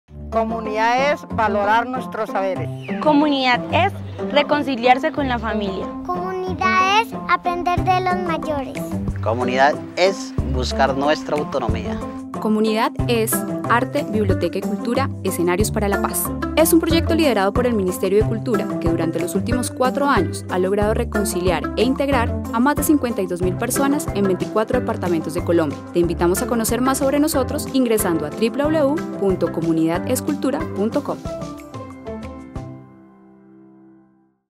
Comunidad-es I Canal Tre3ce
¿Qué es Comunidad-es? Compromiso, amor, respeto, tolerancia, amistad, etc. Hay muchas palabras que pueden estar dentro de este significado, Canal Tr3ce Colombia, le pregunta a las familias que a lo largo de 4 años nos han acompañado en